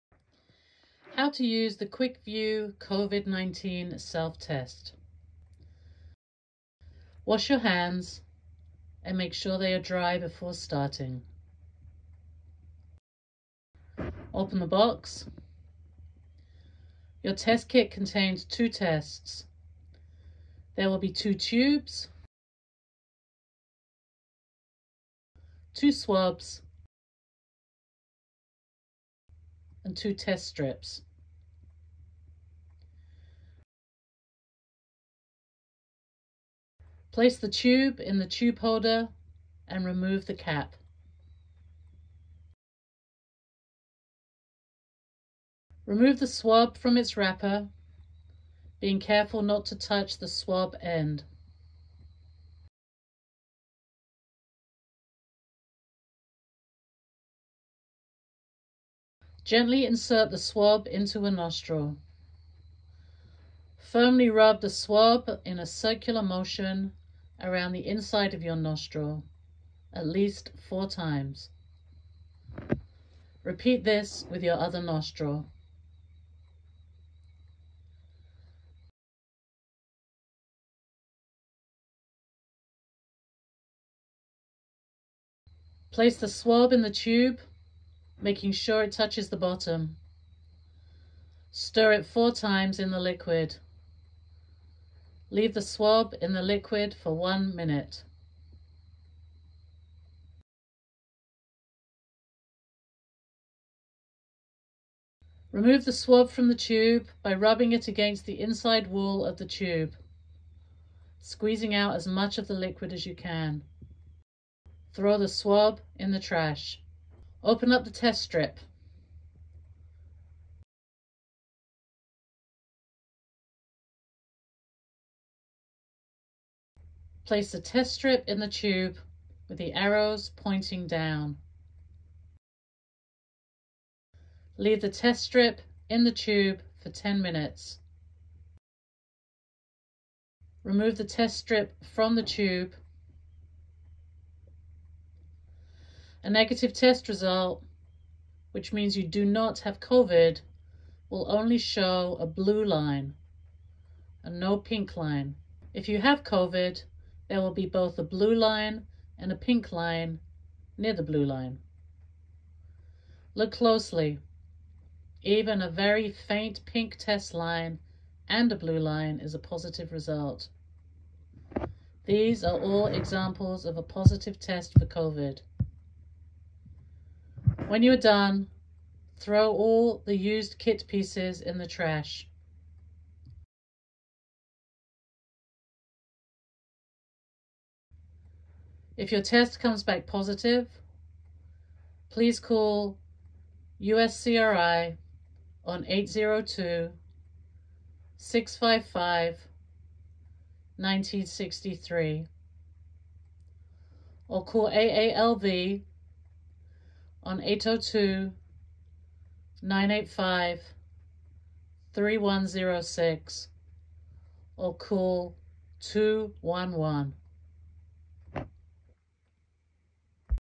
ENGLISH: QUICKVUE rapid antigen self test. How to do it.
How to use the QUICKVUE COVID 19 SELF TEST.
wash your hands and make sure they are dry before starting.
Your test kit contains two tests.  There will be two tubes, two swabs and two test strips
Place the tube in the tube holder and remove the cap.
Remove the swab from its wrapper, being careful not to touch the swab end.
Gently insert the swab into a nostril.
Firmly rub the swab in a circular motion around the inside of your nostril, at least four times.  Repeat this with your other nostril.
Place the swab in the tube, making sure it touches the bottom.  Stir it 4 times in the liquid.
Leave the swab in the liquid for one minute.
Remove the swab from the tube by rubbing it against the inside wall of the tube squeezing out as much of the liquid as you can.
Throw the swab in the trash.
Open up the test strip.
Place the test strip in the tube, with the arrows pointing
down.
Leave the test strip in the tube for ten minutes.
Remove the test strip from the tube.
A negative test result, which means you do not have covid will only show a blue line, and no pink line.
If you have covid, there will both a blue line and a pink line near the blue line.    Look closely, even a very faint pink test line and  a blue line is a positive result.
These are all examples of a positive test for covid.
When you are done, throw all used kit pieces in the trash.
If your test comes back positive, please call USCRI on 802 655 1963, AALV on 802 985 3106 or 211.